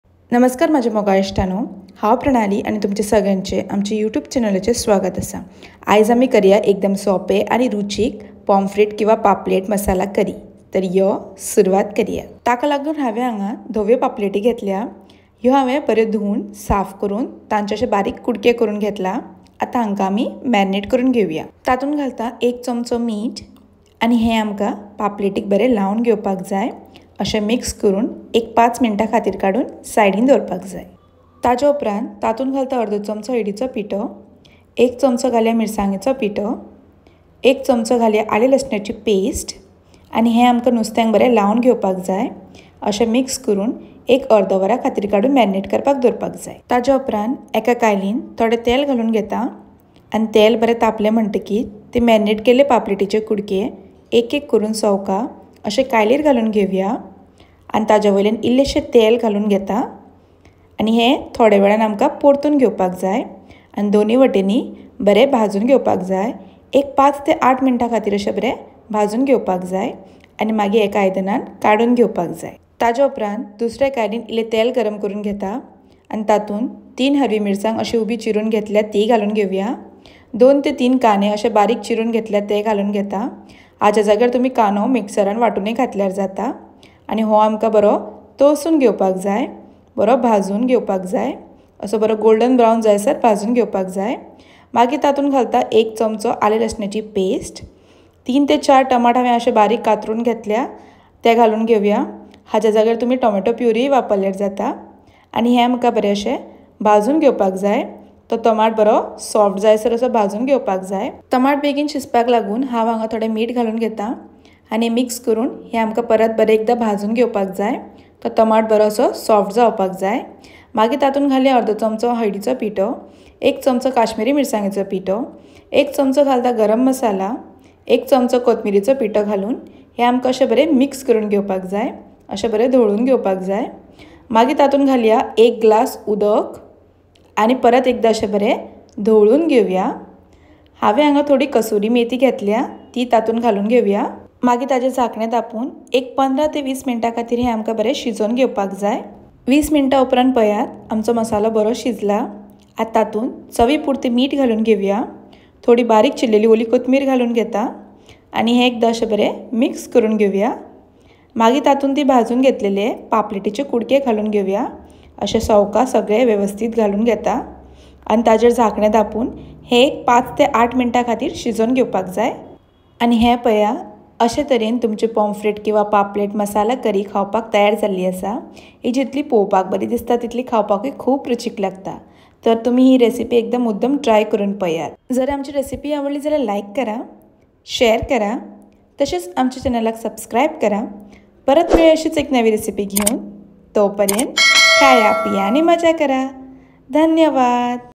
How to Make Pomfret Fish Masala Curry | Paplet Masala curry
Pomfret Masala Curry | Paplet Masala Curry
Ingredients:-
•Fresh Pomfrets (Washed & cleaned)
•1 tsp Salt
•1/2 tsp Turmeric powder
•1 tsp Red chilli powder
•1 tsp Ginger Garlic paste
•Cooking Oil
•3 Green chillies slit
•2-3 Onions
•1 tsp Ginger Garlic paste
•3-4 Tomatoes
•1/2 tsp Turmeric powder
•1 tsp Kashmiri Red chilli powder
•1 tsp Garam Masala
•1 tsp Coriander powder
•1 glass Water
•Kasuri Methi (Optional)
•Salt as per taste
•Coriander Leaves
Also Comment down which recipe you want me to try next✅
How to make Pomfret Masala Curry, Paplet masala curry recipe, Goan Fish Masala Curry, Fish masala curry recipe, Recipe in konKani Cuisine